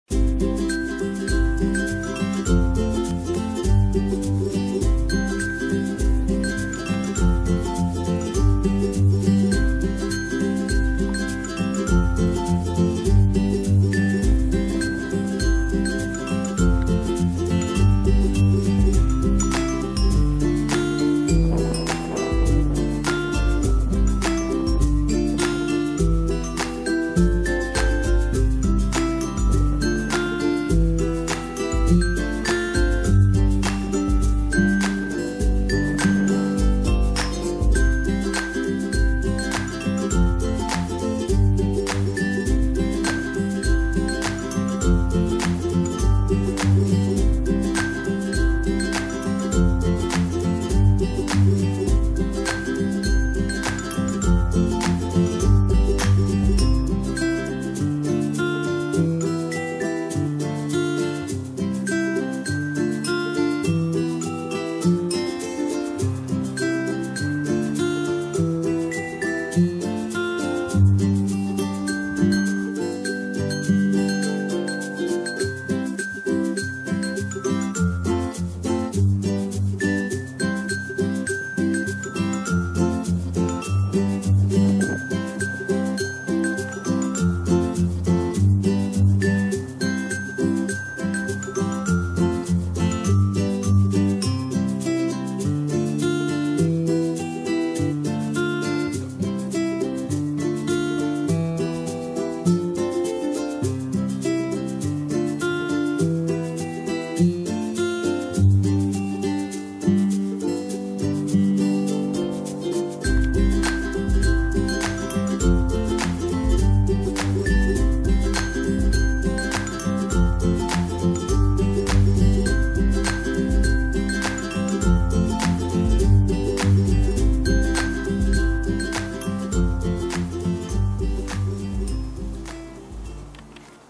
Horses for Kids - Horse Videos for Children
A fun video with a lot of horses! Watch these animals now.